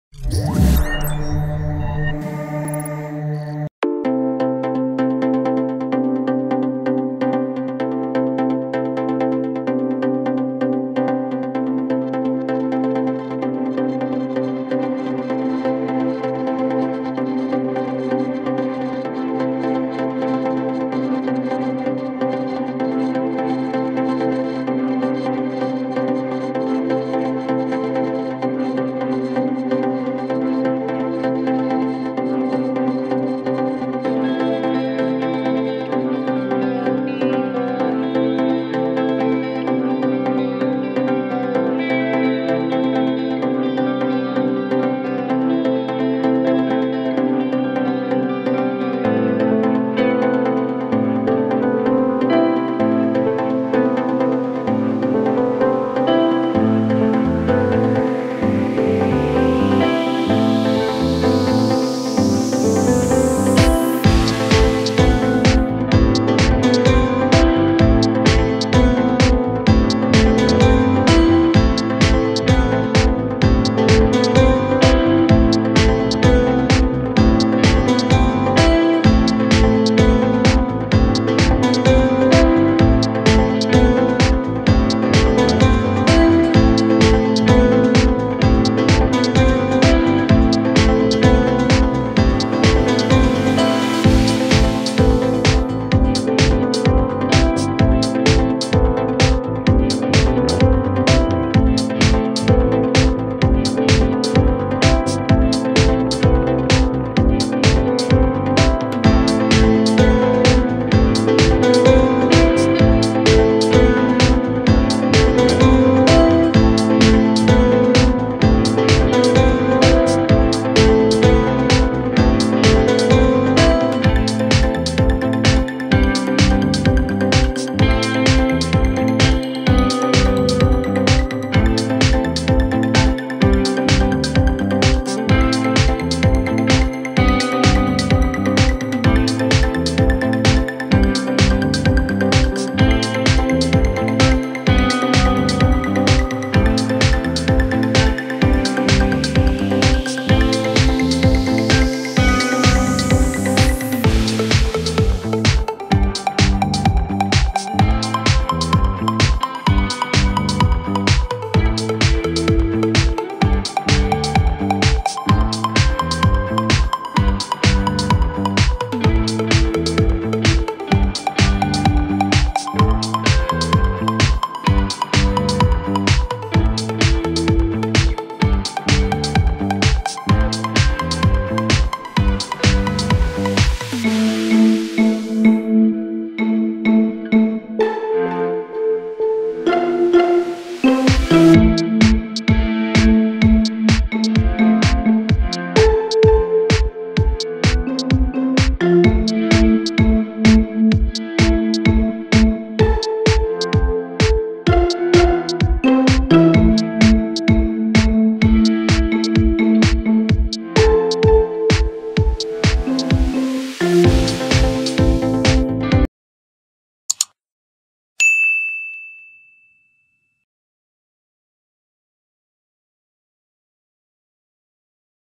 조보아(JOBOA), 바람에 미모가 날린다! - RNX tv
배우 조보아(JOBOA)가 9월 18일 오후 인천 중구 운서동 인천국제공항을 통해 해외 화보 촬영 일정 차 태국으로 출국했다.  [서울=RNX뉴스] 이성해 기자